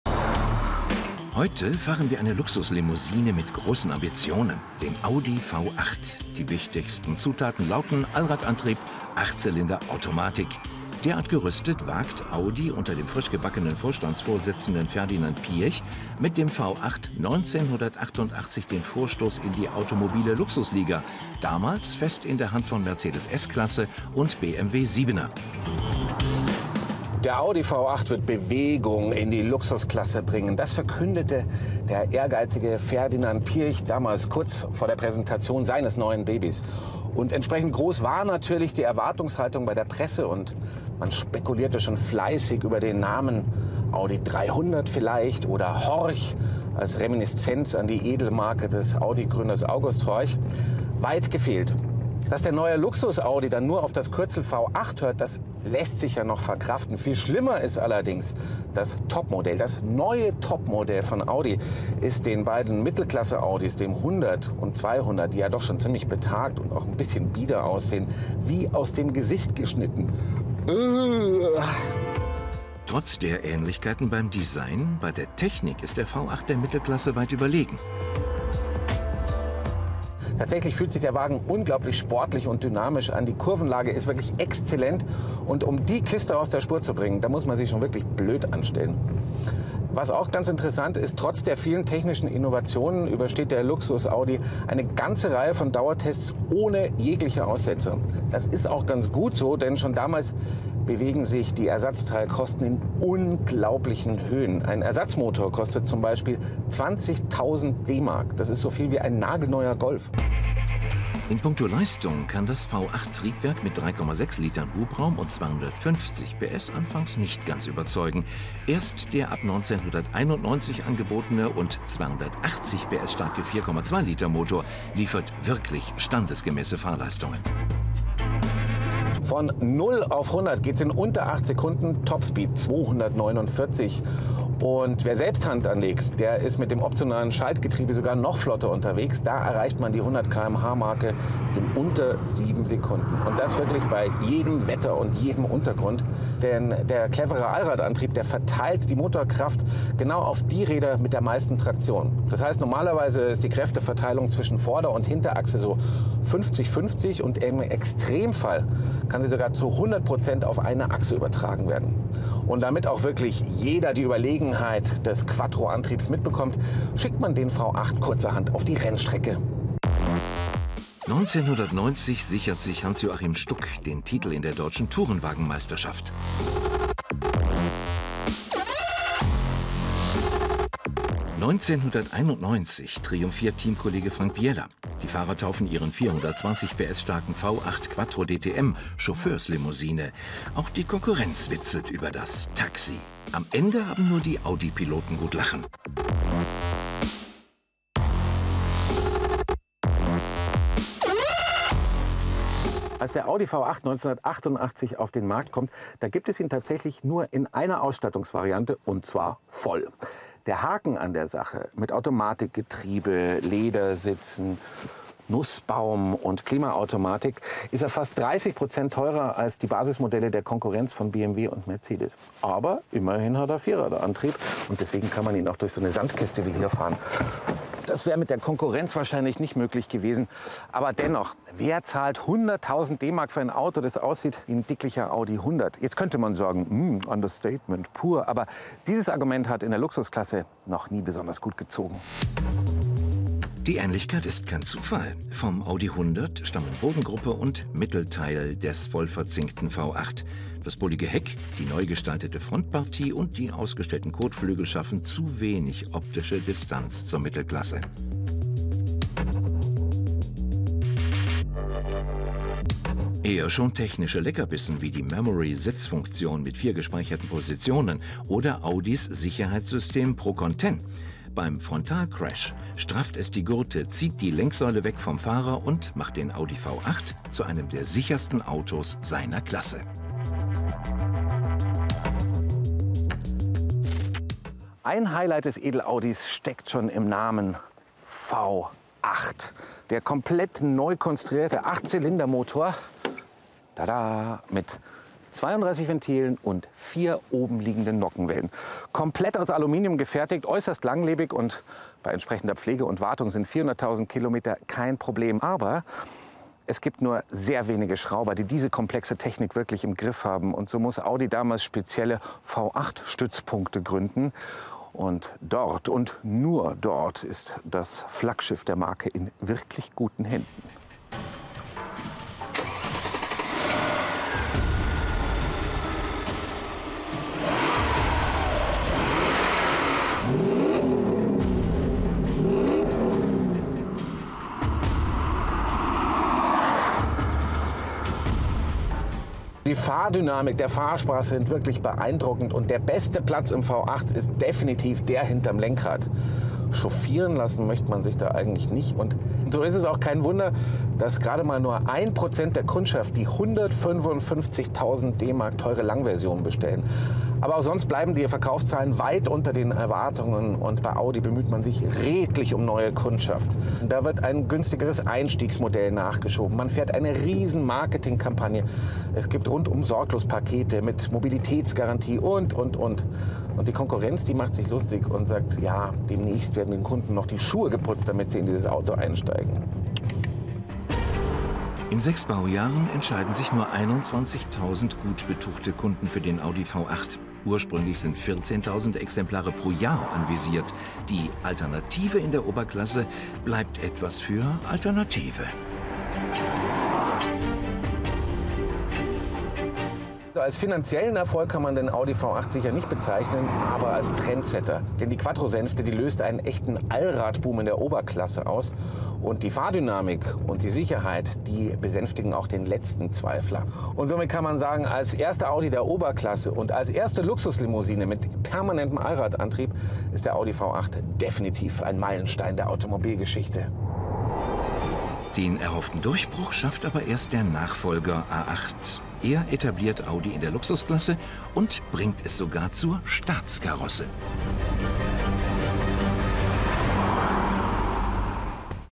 Damals revolutionär: Audi V8 | DW Deutsch
Die wichtigsten Zutaten des neuen Ingolstädter Flaggschiffs lauten 1988: Allradantrieb, Achtzylinder, Automatik. Derart gerüstet, wagt Audi unter dem frisch gebackenen Vorstandvorsitzenden Ferdinand Piëch den Vorstoß in die automobile Luxus-Liga.